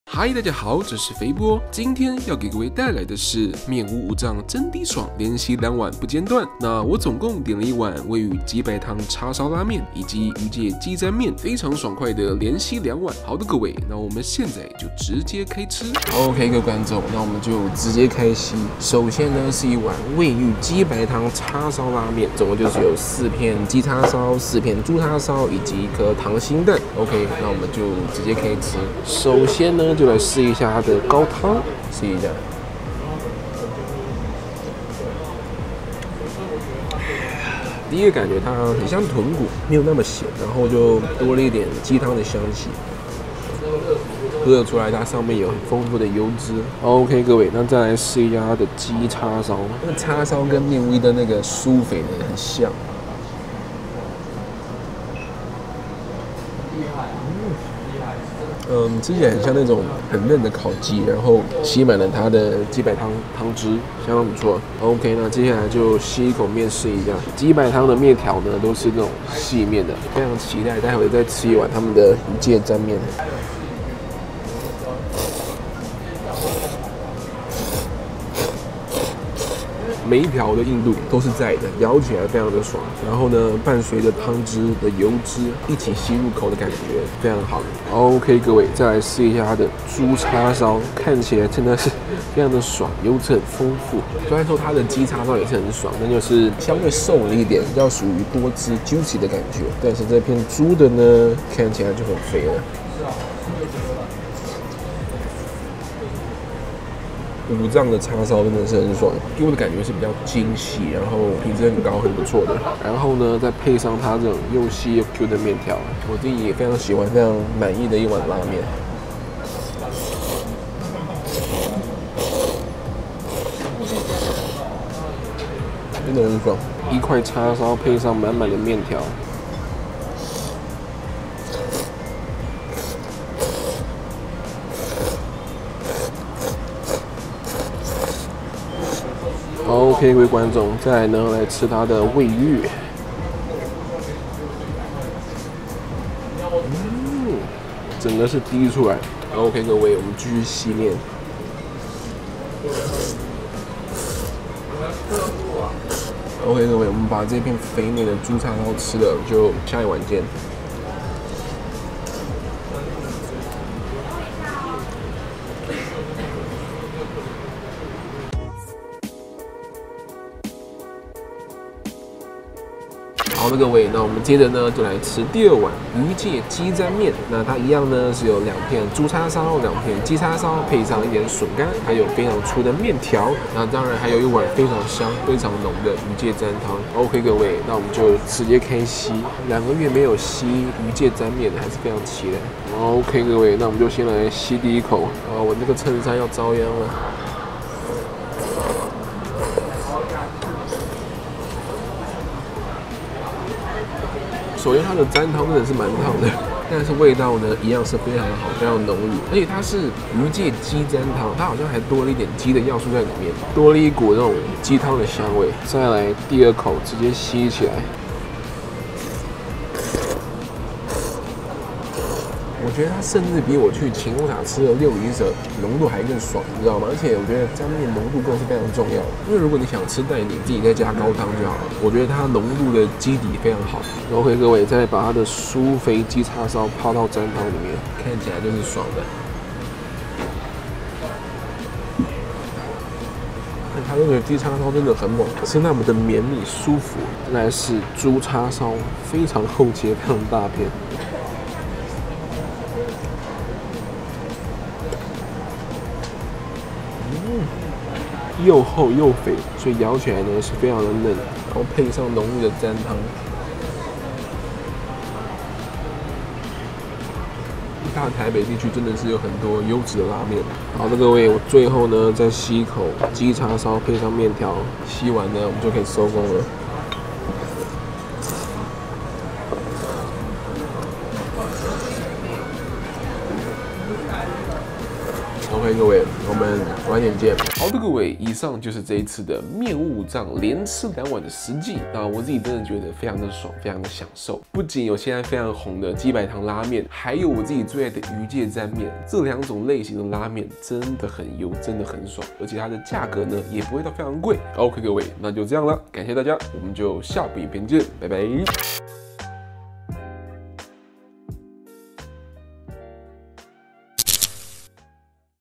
麵屋武藏真d爽，連吸兩碗不間斷 ! 味玉雞白湯叉燒拉麵 + 魚介雞沾麵 【 肥波外食記 】
好麵好麵，上次回去拍的